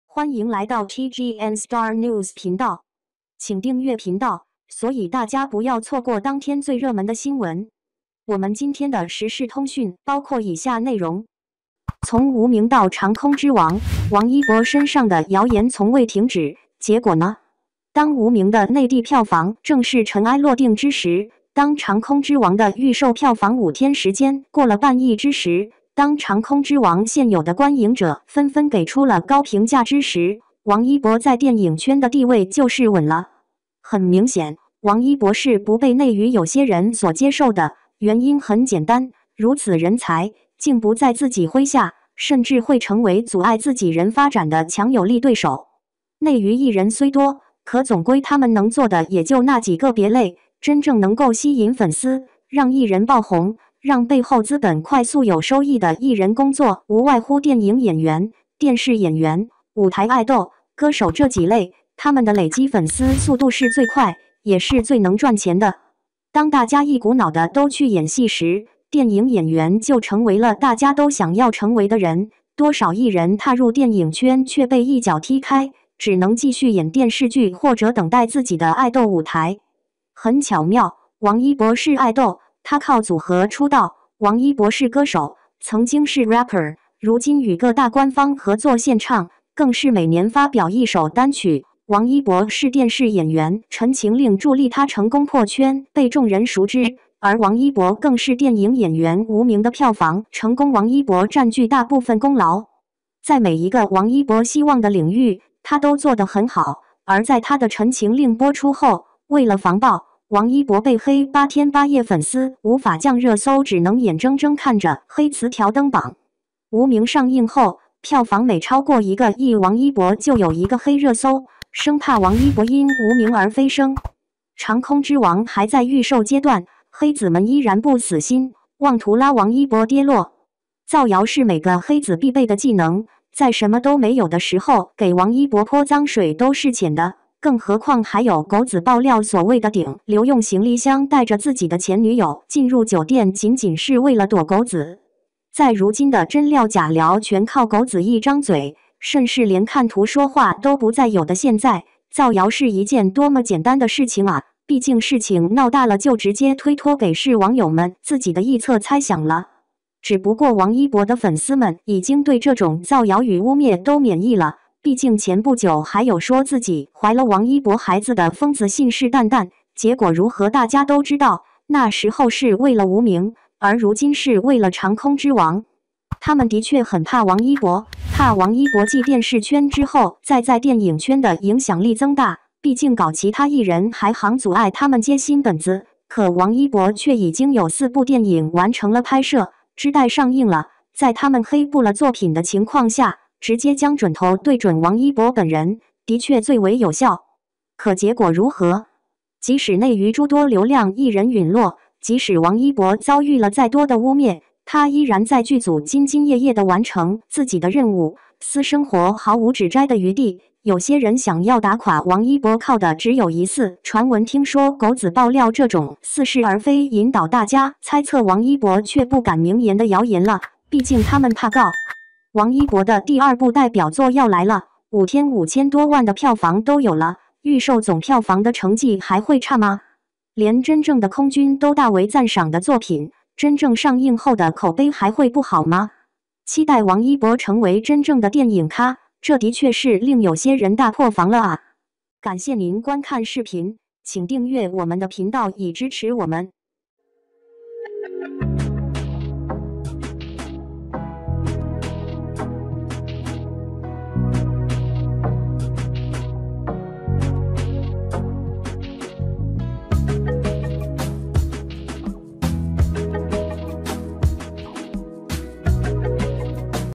从《无名》到《长空之王》，王一博身上的谣言从未停止，结果呢？
从《无名》到《长空之王》，王一博身上的谣言从未停止，结果呢？ | TGN Star News